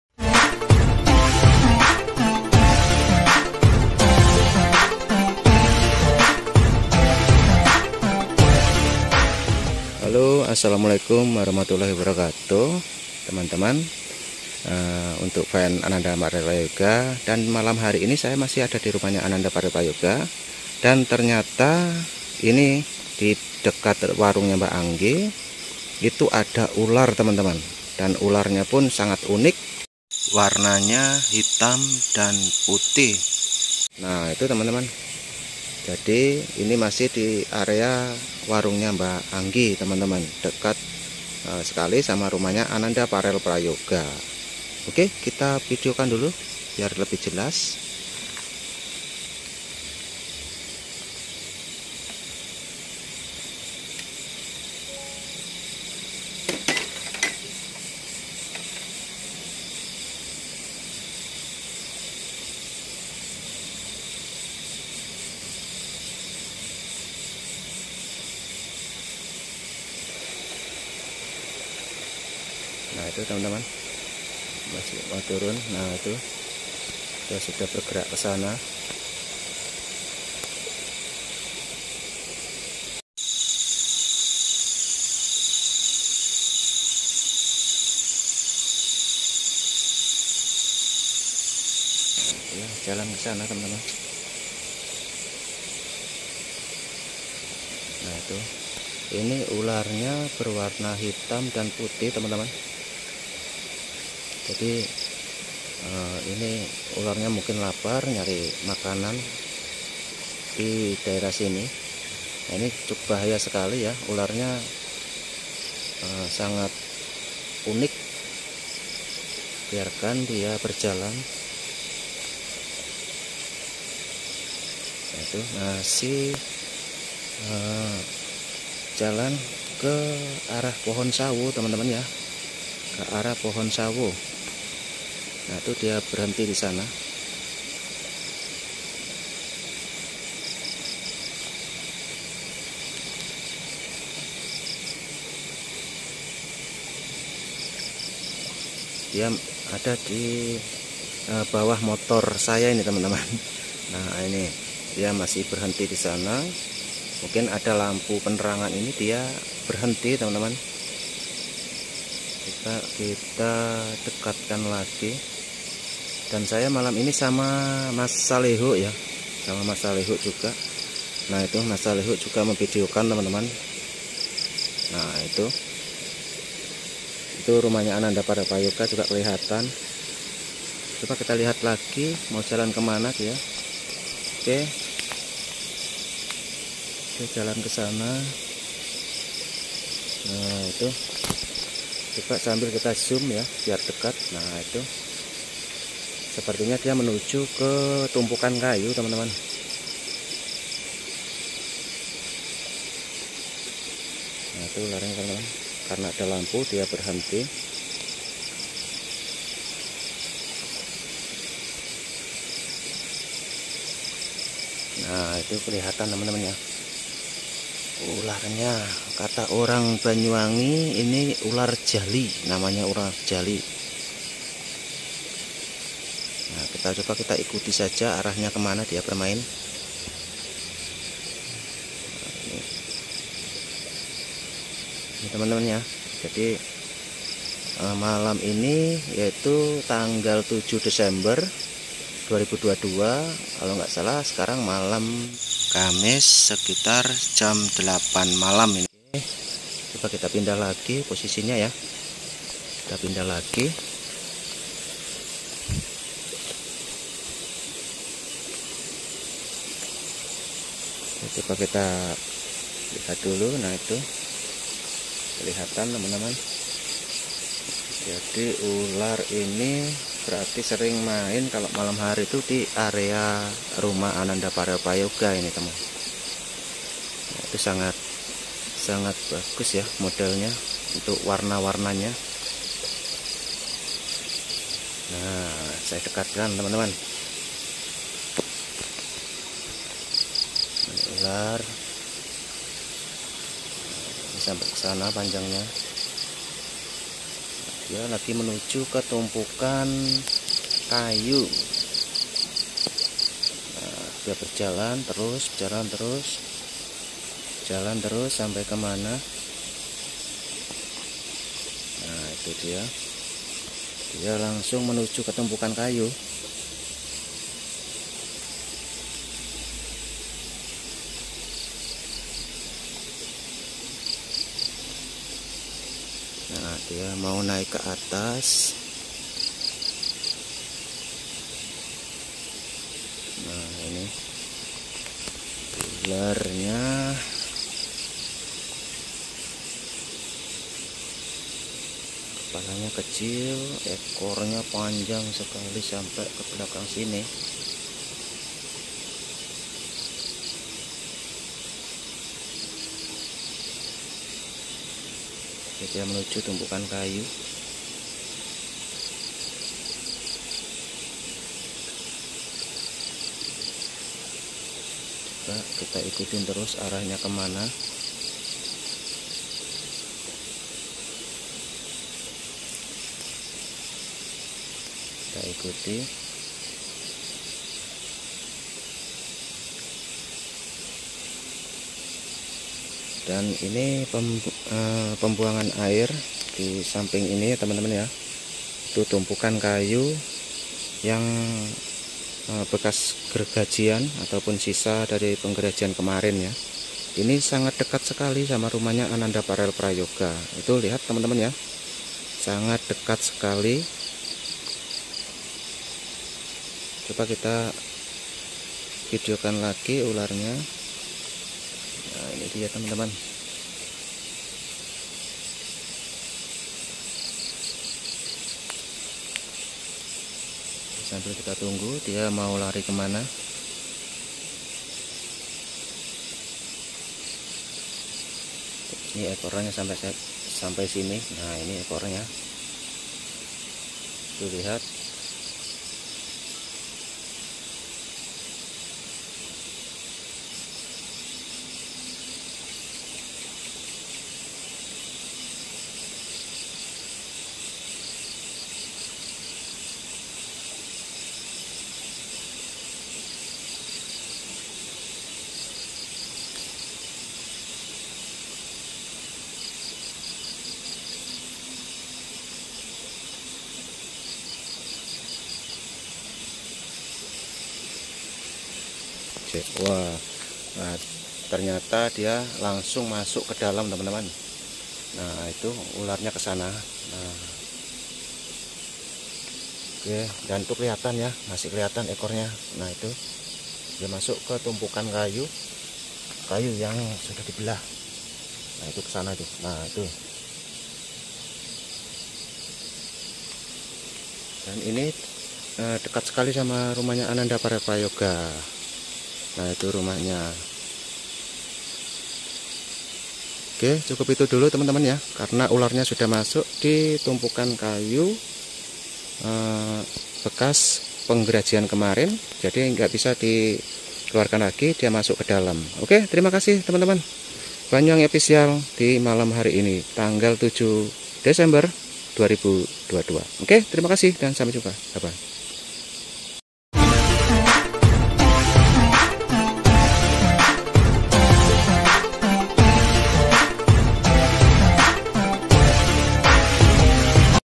youtuber belum tau‼️Di depan rumah farel Prayoga ada ular || farel viral
youtuber belum tau‼️Di depan rumah farel Prayoga ada ular || farel viral,di malam ini ada ular weling di depan rumah farel Prayoga,ular nya warna hitam dan putih,dan tidak ada yang tau 😂